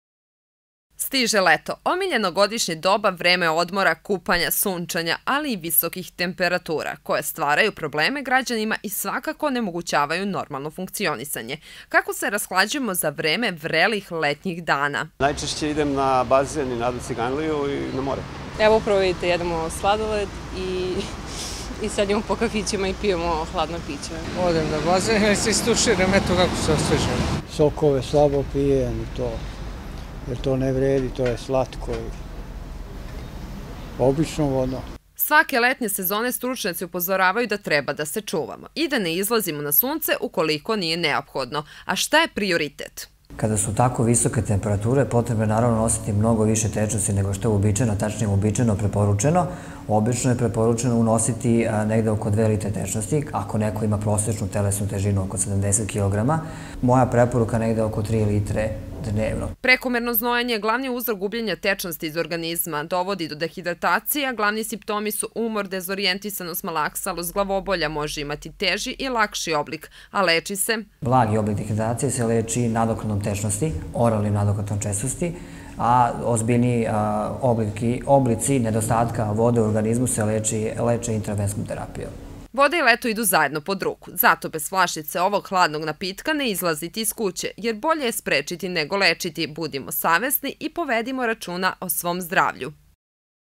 Unos tečnosti tokom leta
Tokom letnjih meseci pozivi hitnoj pomoći su najčešće upućeni zbog dehidratacije otrganizma. Istraživali smo kako se staropazovčani razhlađuju na visokim temperaturama i šta savetuju lekari koliko nam je tečnosti potrebno za vreme tropskih dana?